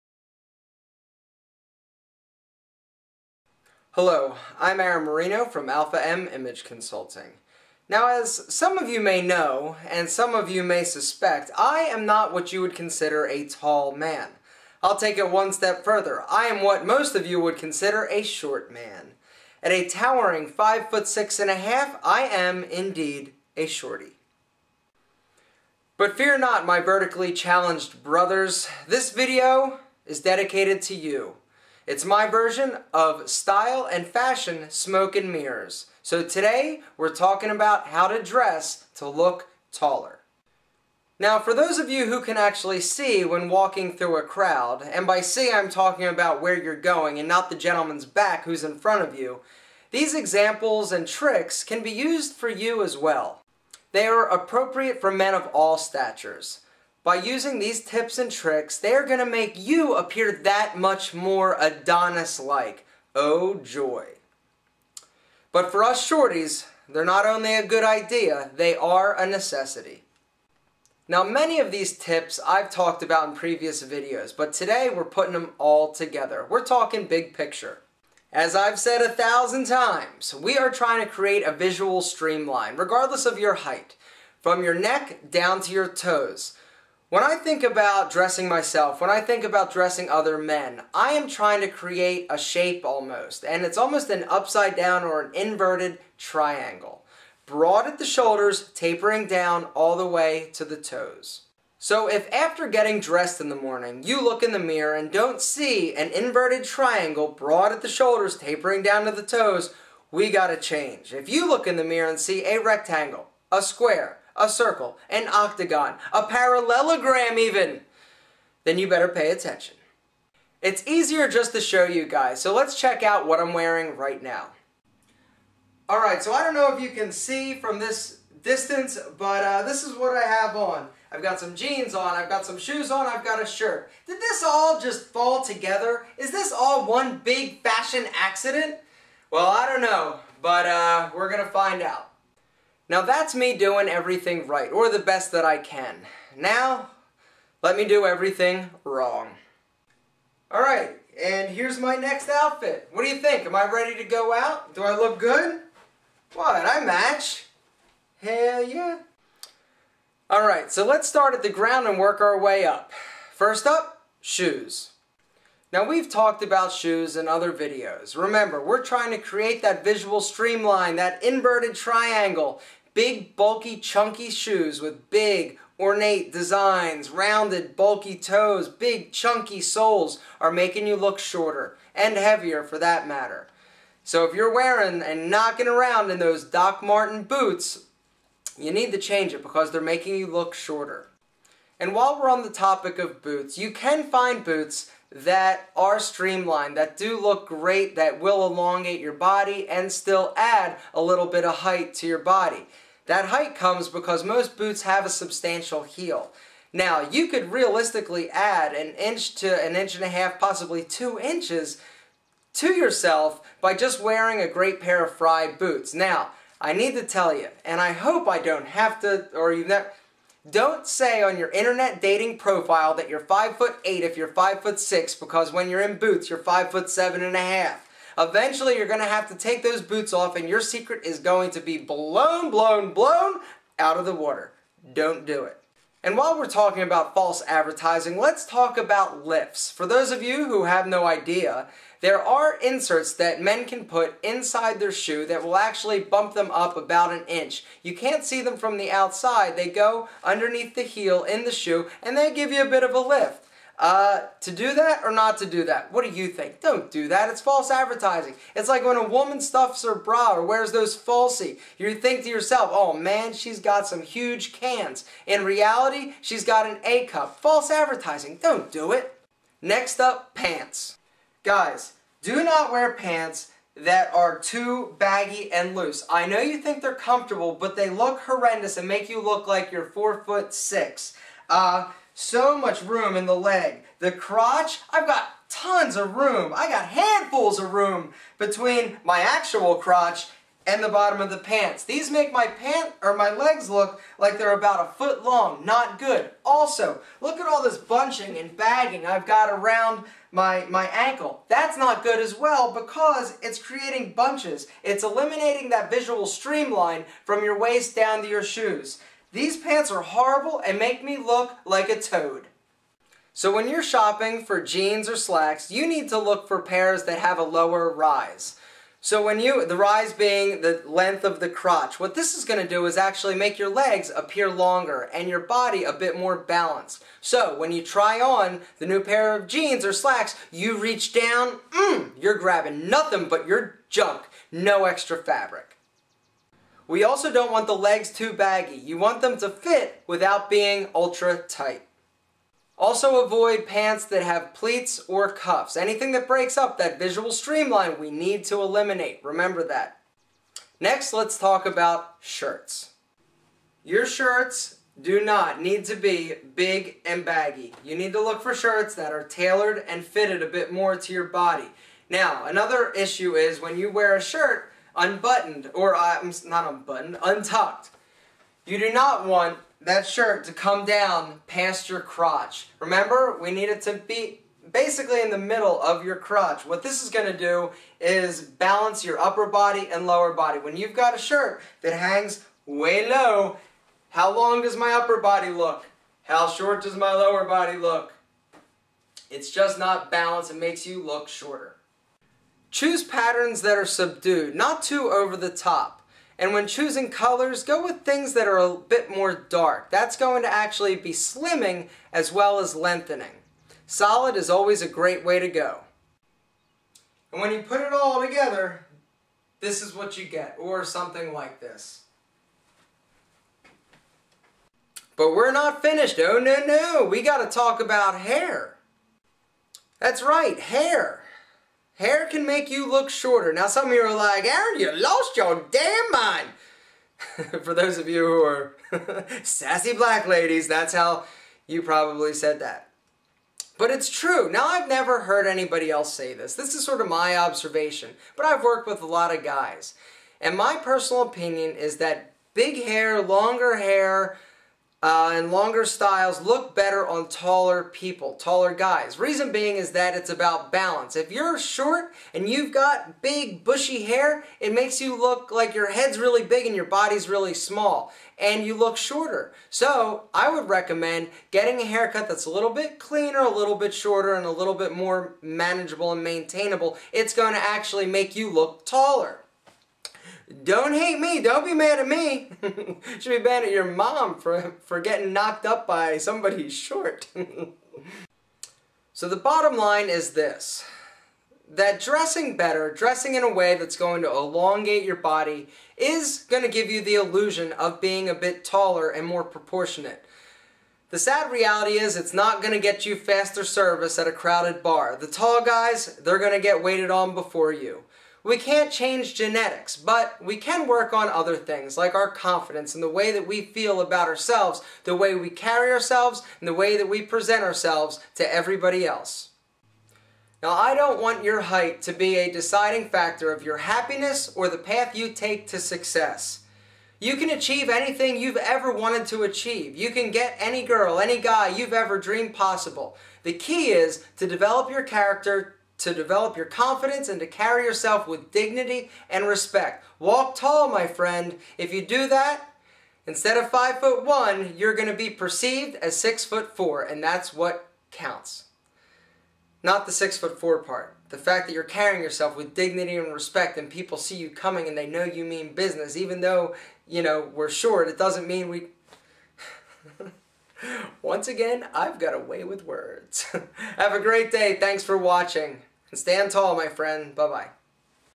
Dressing to Look Taller: Fashion Smoke and Mirrors!
Men's Style, Grooming and Fitness
Professional male image consultant and fashion expert, Aaron Marino, shows you how to look taller with the help of proper clothing choices.  Being short doesn't have to be a curse, if you know how to dress.  Appearing taller is not as hard as you may think.